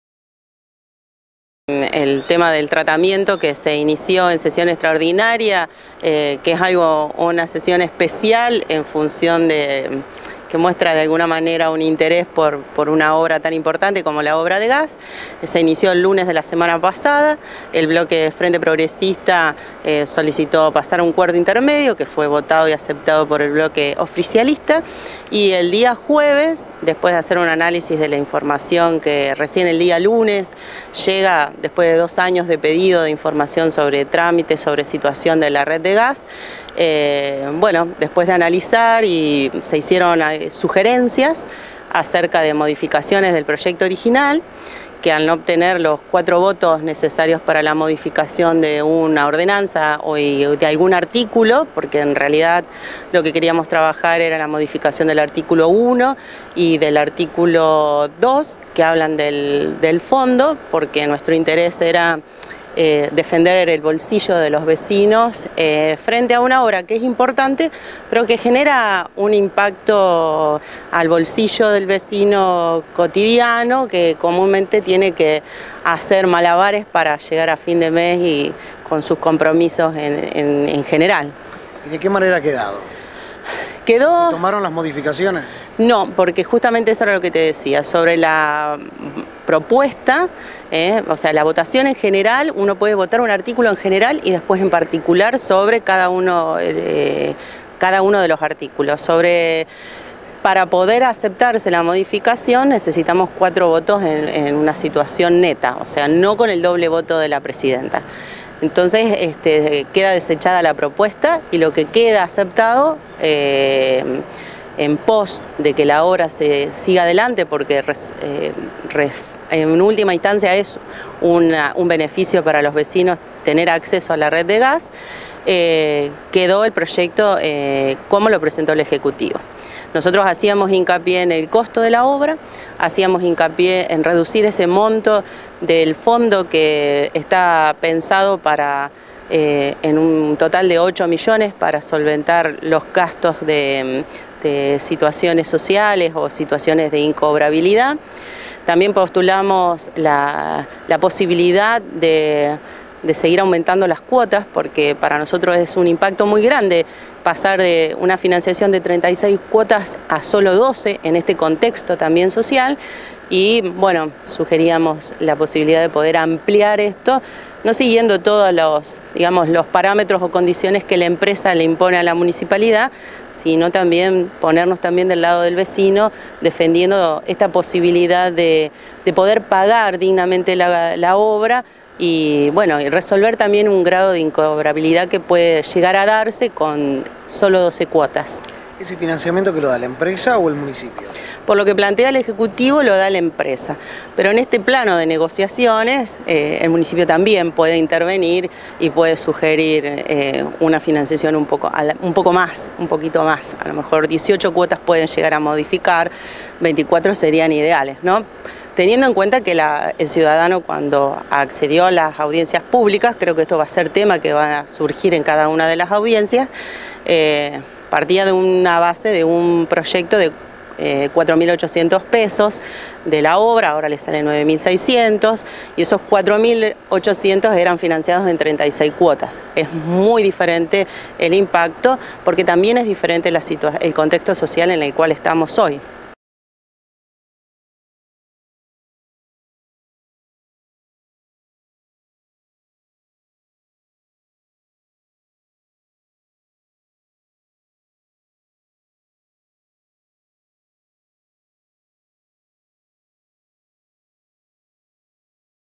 Red de gas en Roldán: el Concejo aprobó proposición de la Municipalidad
Se reactiva la obra de gas natural en Roldán con el visto bueno brindado por el Concejo Municipal a la Intendencia.